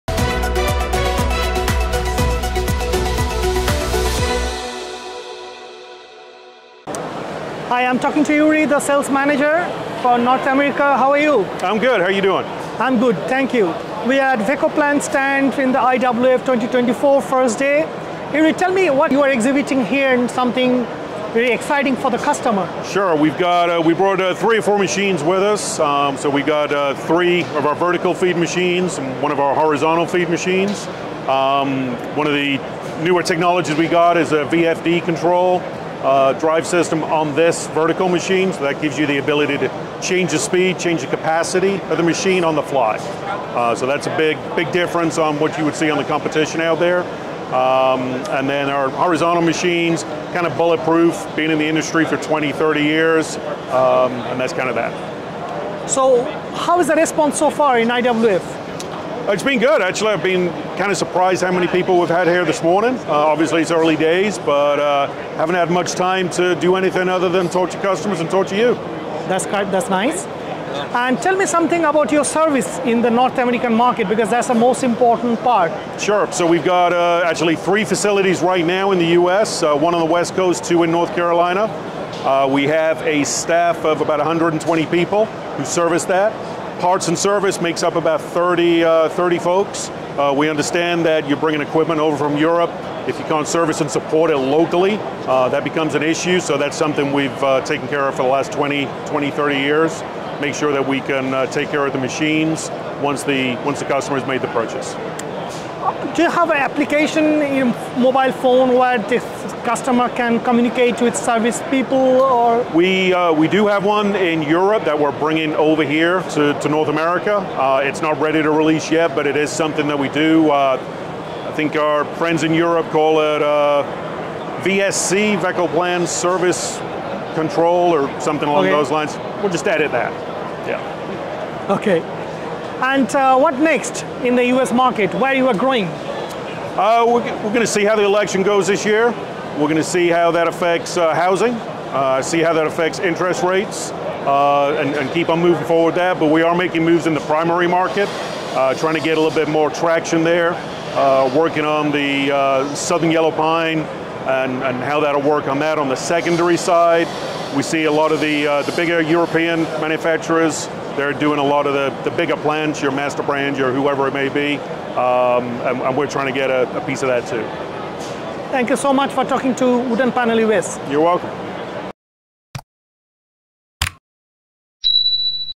Vecoplan gets the most out of everything at IWF Atlanta
In an exclusive interview with Apratim Ghoshal, Editor-in-Chief, Yuri Chocholko, Sales Manager, Wood, Biomass and Biofuels shares his excitement about being present at IWF, highlighting the cutting-edge innovations and can streamline your workflow and enhance production and more.